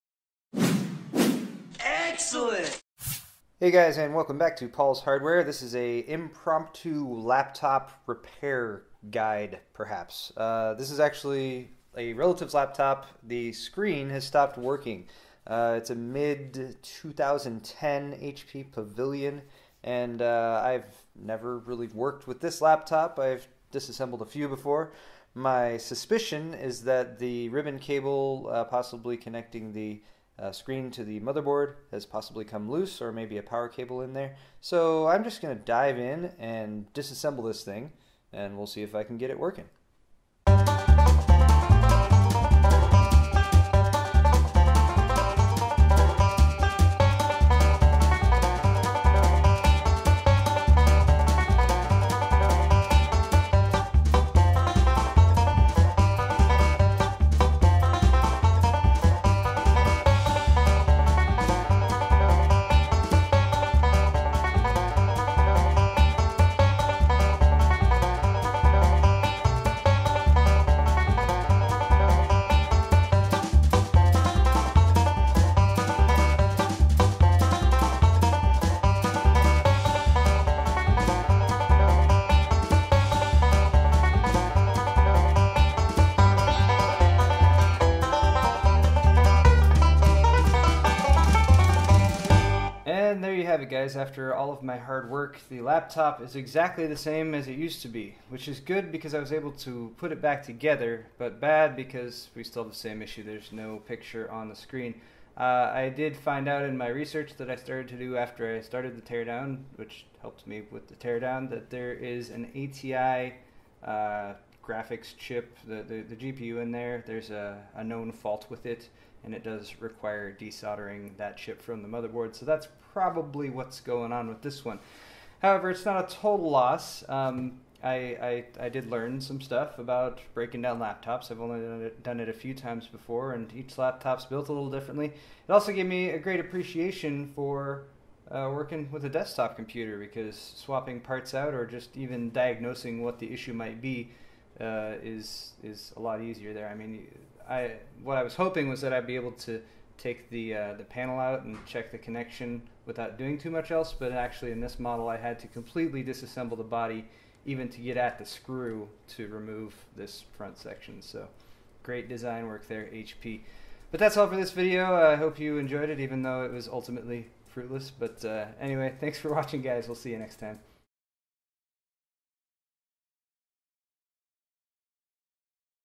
How Not To Repair a Laptop
I try, and fail, to repair an HP laptop whose screen won't show a picture.